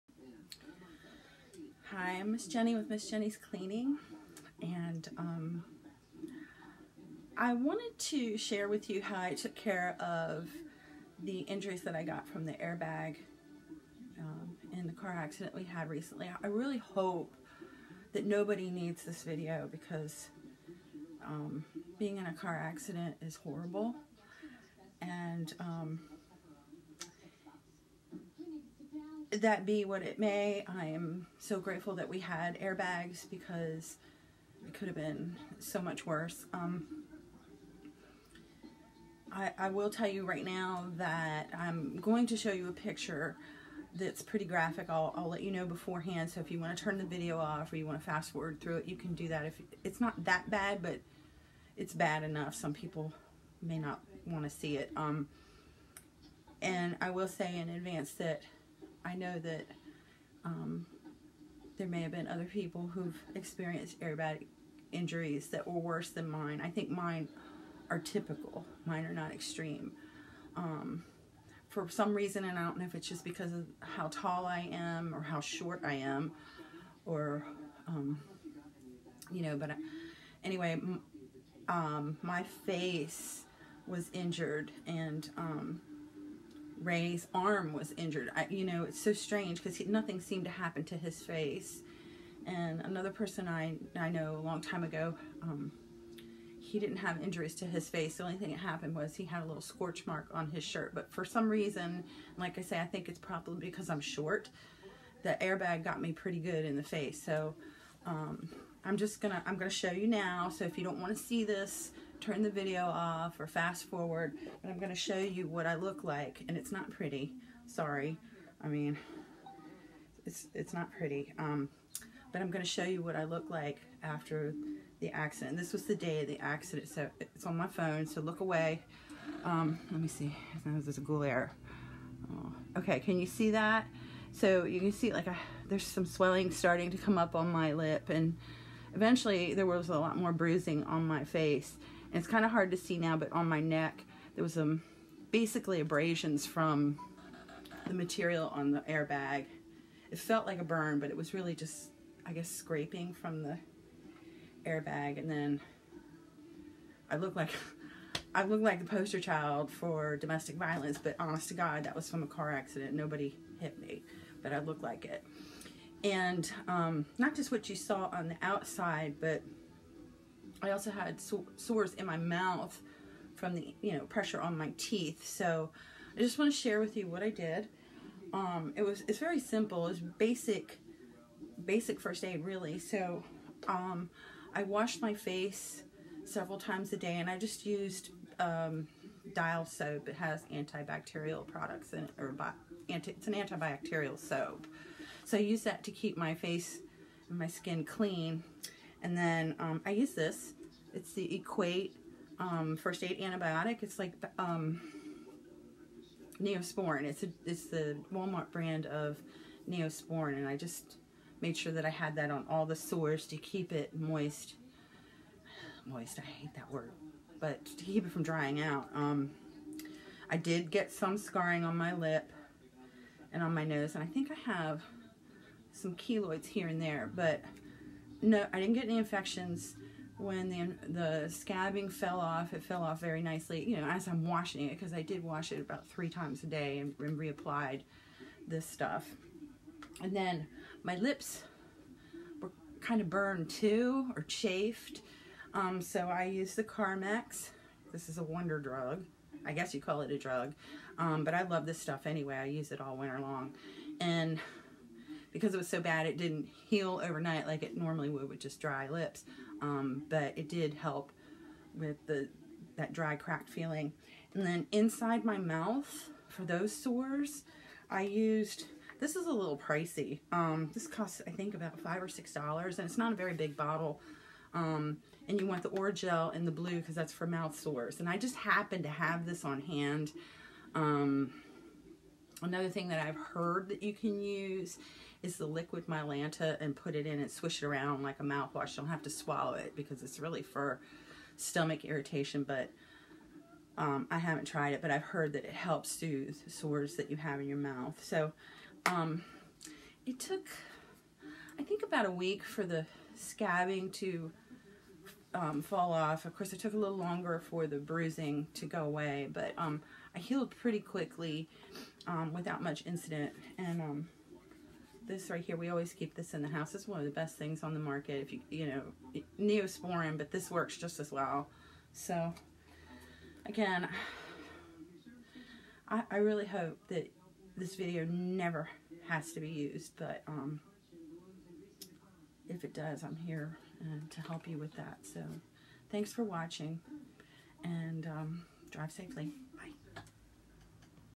Airbag Injury First Aid (Slightly Graphic)
I hope you never need this information. But if you do, I hope you find this video helpful.

Please drive safely!

Products:
Carmex Original Flavor Moisturizing Lip Balm
Dial Soap
Equate Triple Antibiotic First Aid Ointment, 1 Oz Tubes
Orajel Antiseptic Mouth Sore Rinse 16 oz
Mylanta Maximum Strength Antacid

How To Clean Windows And Mirrors Like A Pro- Streak Free Method To Make Glass Shine

*This video is not sponsered*